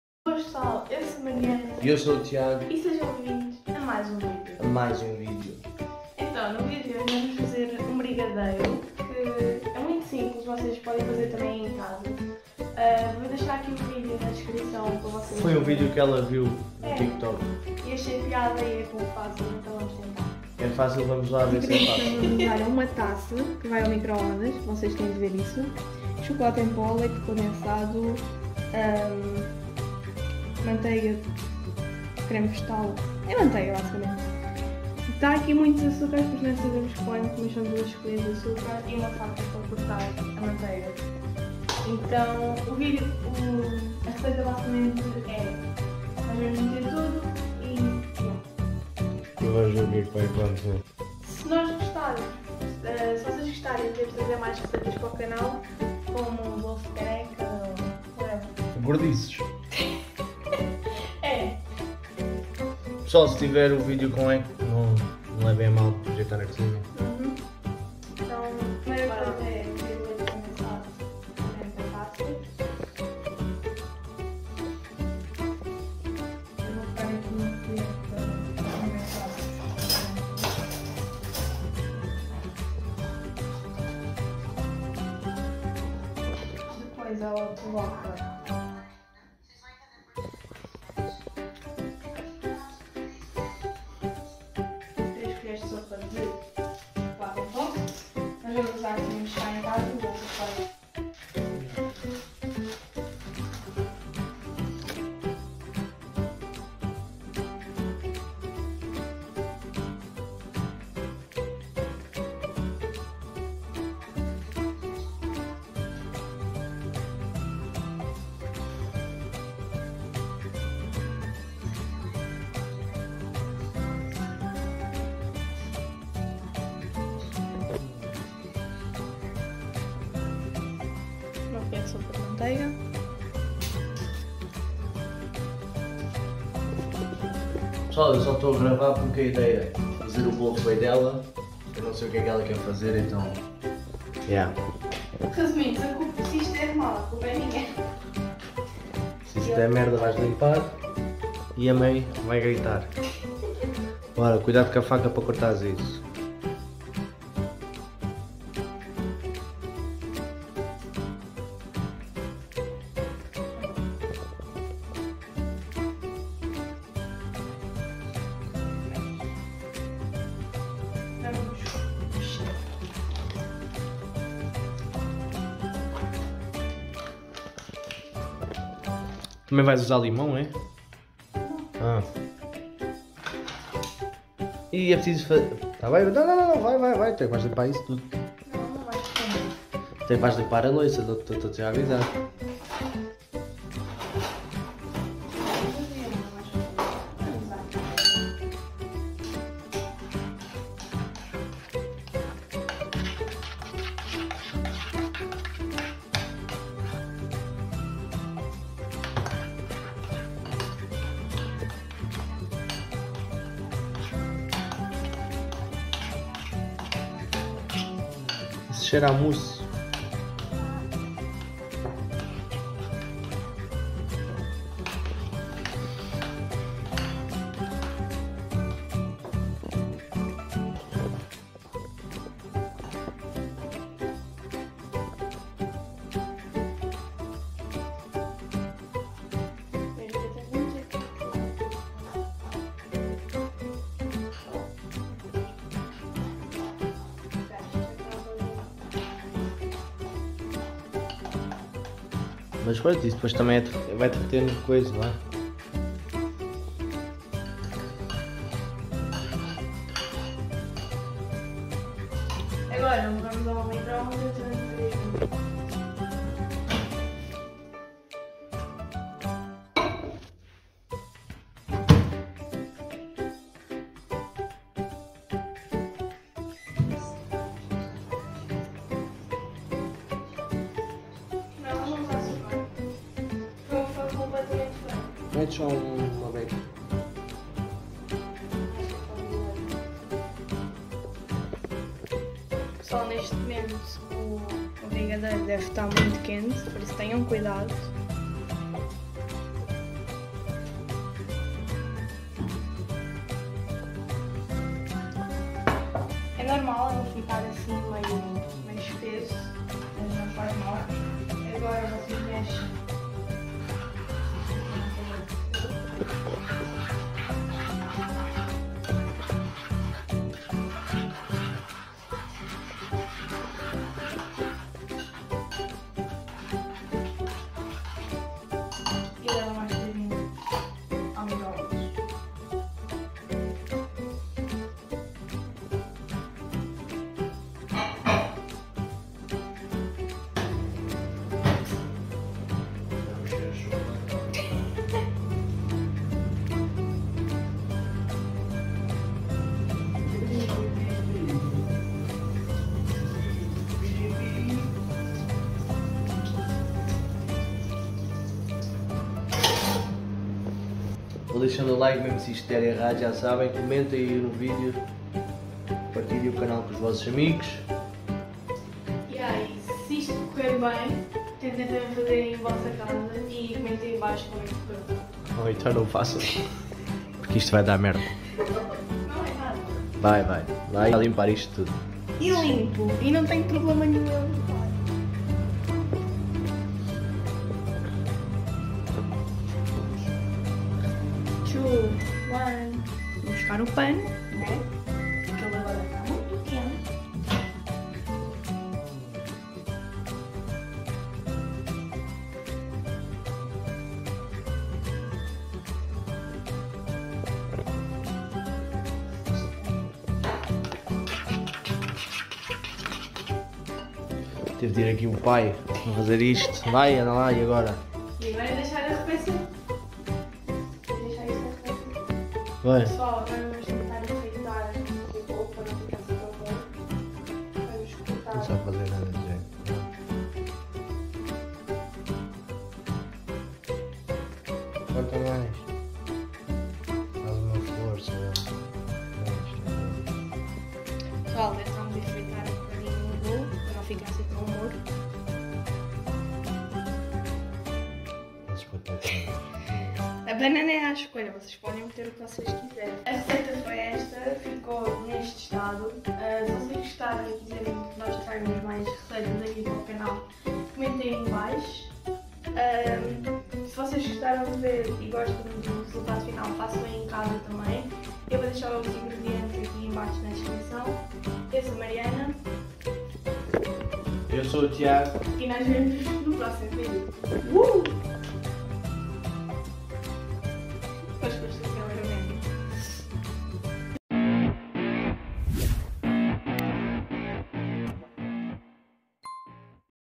VLOG #3 -Um dia na cozinha (DEU RUIM!!!)
INGREDIENTES:
-Leite condensado
-Chocolate em pó
-Açucar
-Manteiga

Subscrevam o canal 🤙🏼
Comentem ideias de vídeos 🤙🏼

Mariana
Facebook Mariana Valente